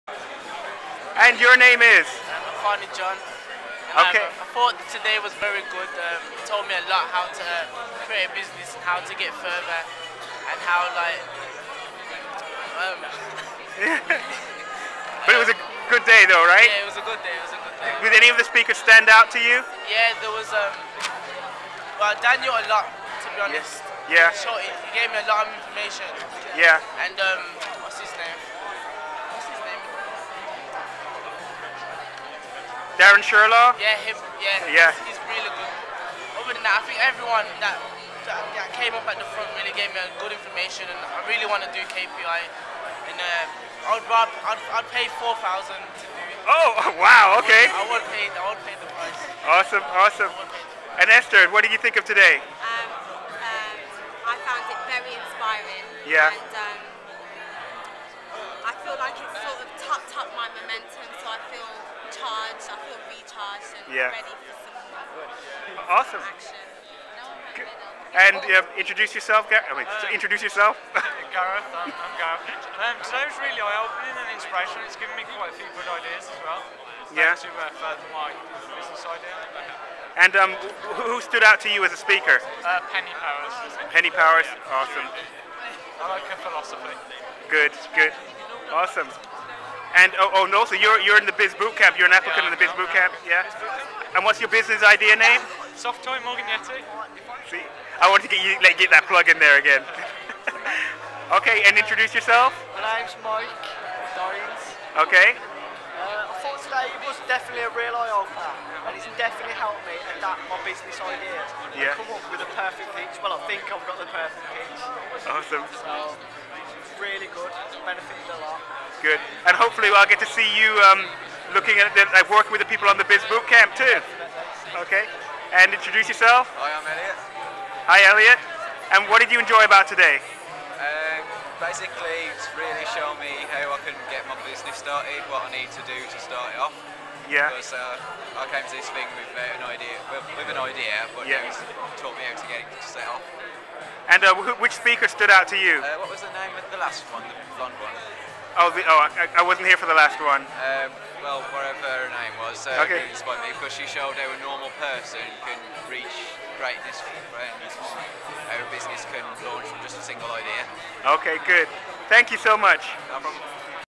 Birmingham KPI's of the Future
Thoughts on the Becoming A Key Person of Influence one day event 26th March.  Akanni, Esther, Gazz, Mike and Elliot